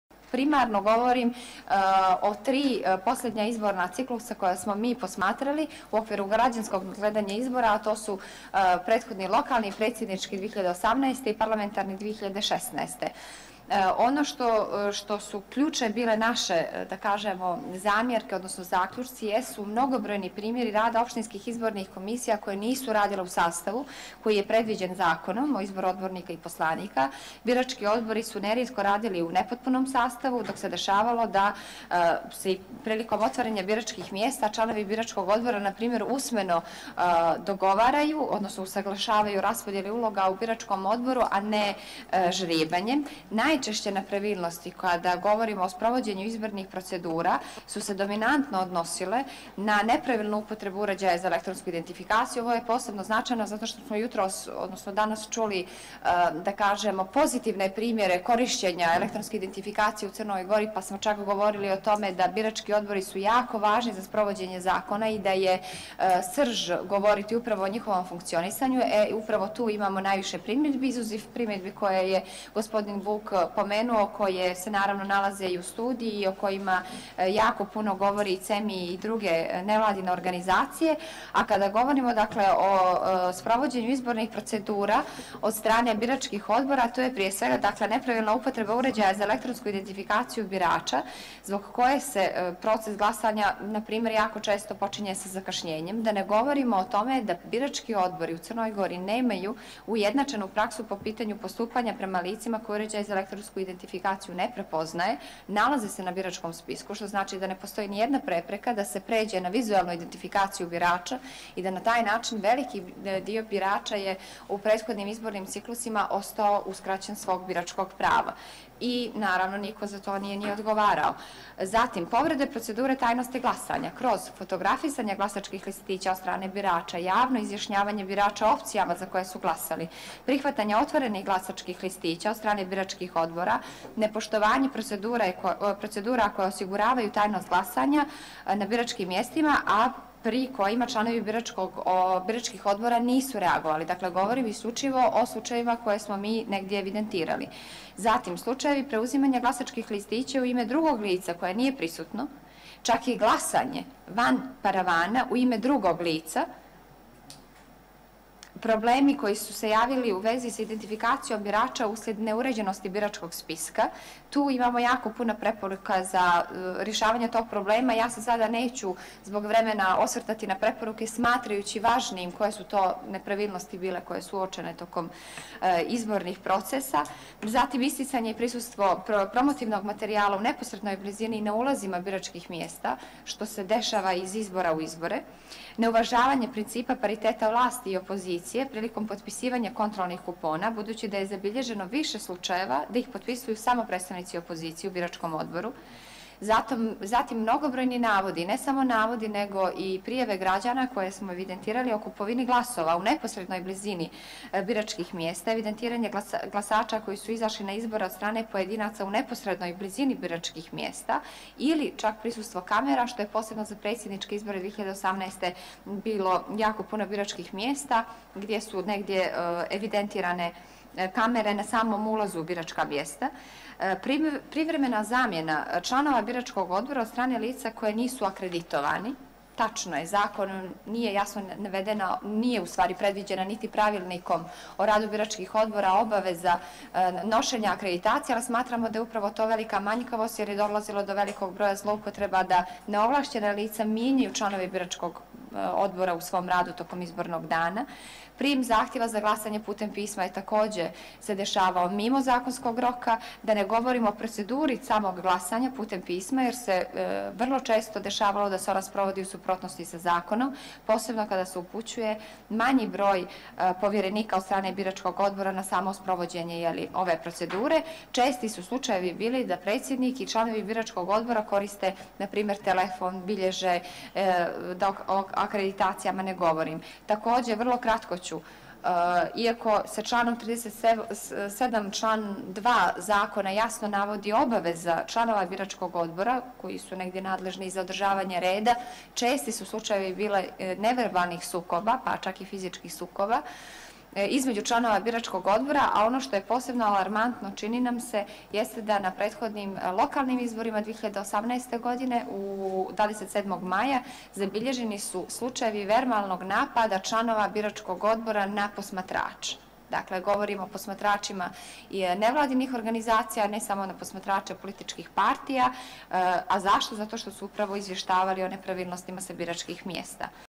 Nikoleta Đukanović, izvršna direktorica CeMI-ja I 10.07.2019.
Profesionalizacija Državne izborne komisije (DIK) bi uticala na njen ozbiljniji i profesionalniji rad, a potrebno je i obezbijediti pristup medijima prilikom održavanja sjednica DIK-a, čime bi rad te institucije bio transparentniji.

To je saopštio istraživač javnih politika u Centru za monitoring i istraživanje (CeMI), Nikola Zečević, u drugom dijelu nacionalne konferencije koju je CeMI organizovao u okviru projekta „Neka slobodni izbori postanu navika – Izgradnja povjerenja u integritet izbornog procesa u Crnoj Gori“, koji finansira Evropska unija, a kofinansira Ministarstvo javne uprave...